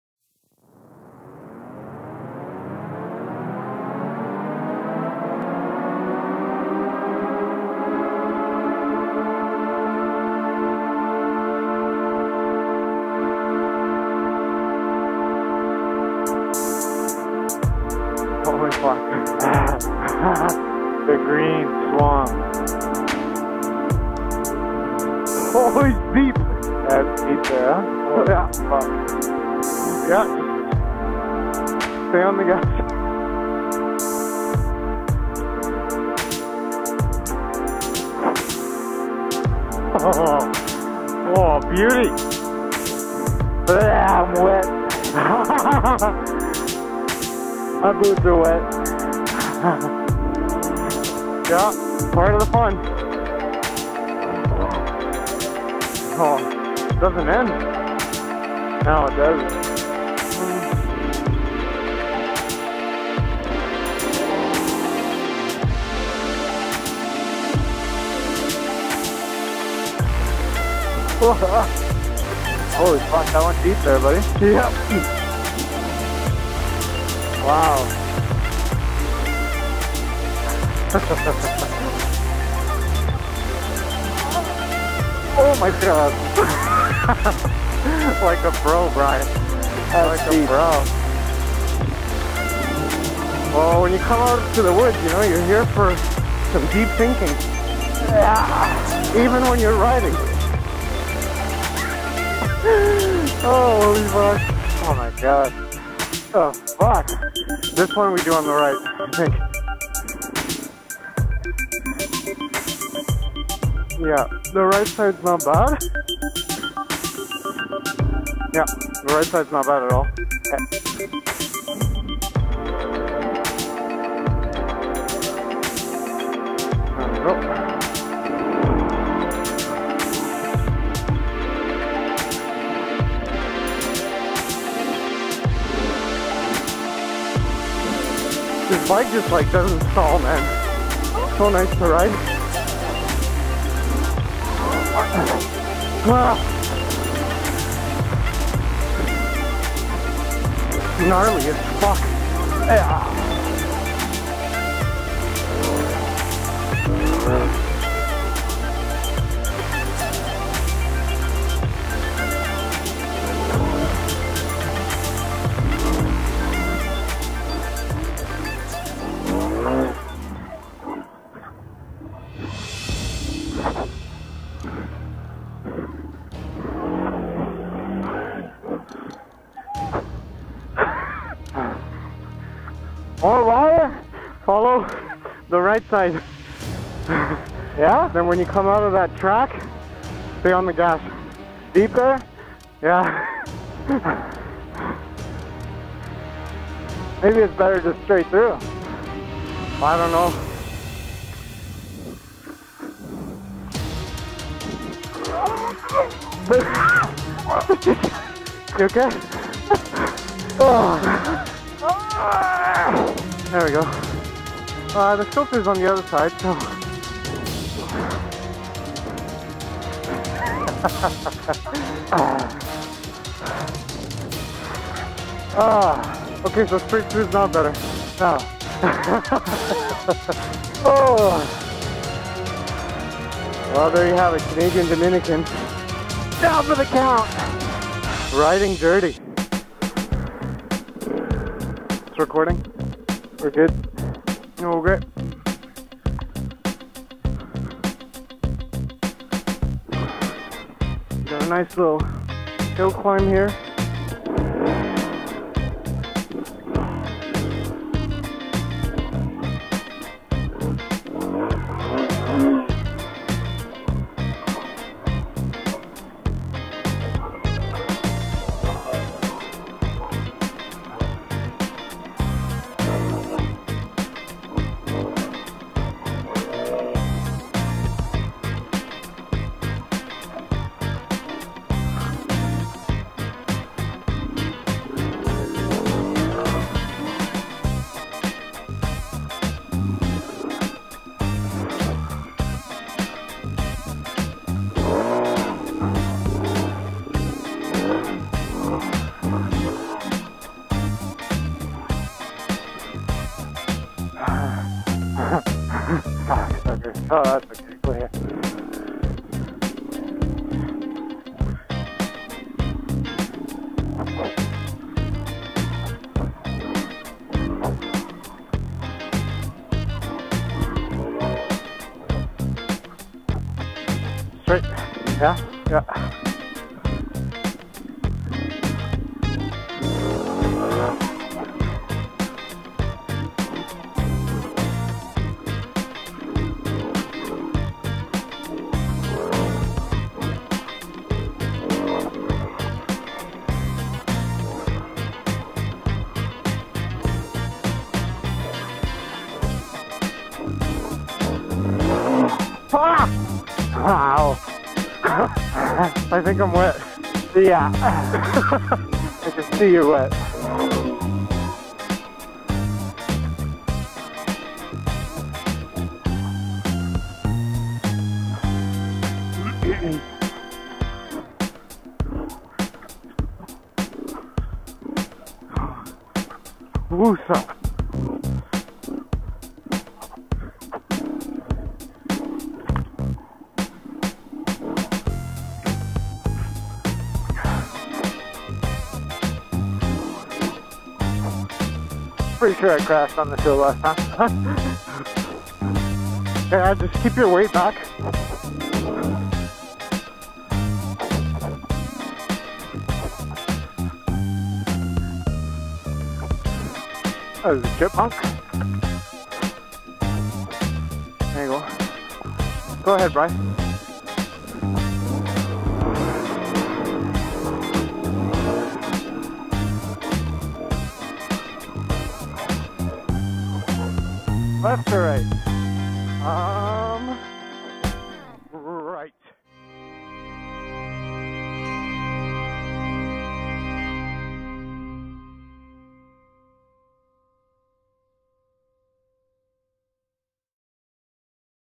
2 Yamaha TTR230s - Enduro mud & swamp crossings with dirt bikes!
Two friends on Yamaha TTR-230s, riding enduro trails, crossing swamps and muddy waters in Calabogie.  Starring Romanian Tractor and Canadian Dominican.  Dirt bike riding is always an adventure with at least one mishap or two.

Romanian Tractor
Shot with GoPro Hero3+ Black
Audio from Sena Bluetooth Pack

In this video:
Canadiandominican